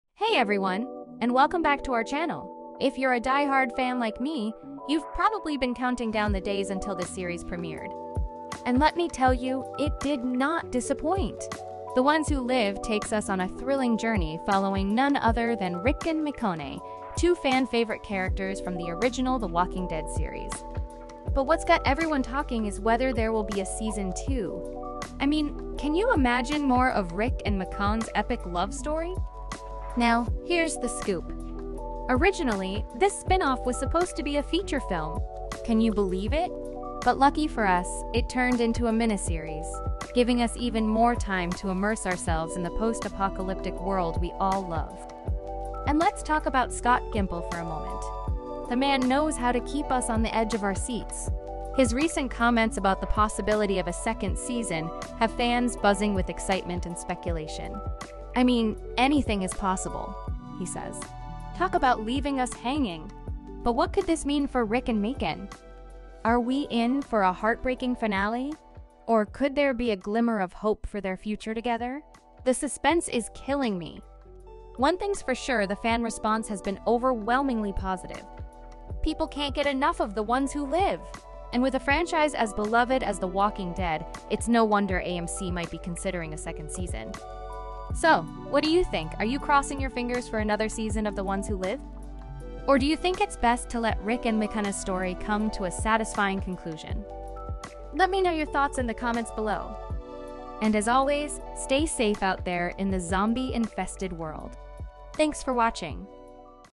The Walking Dead: Possibility of season 2???
Join us as we delve into the gripping world of The Walking Dead: The Ones Who Live, exploring the possibility of a Season 2, dissecting showrunner Scott Gimple's cryptic hints, and capturing the electric excitement of fans worldwide. Get ready for speculation, insights, and all the latest updates on Rick and Michonne's epic journey!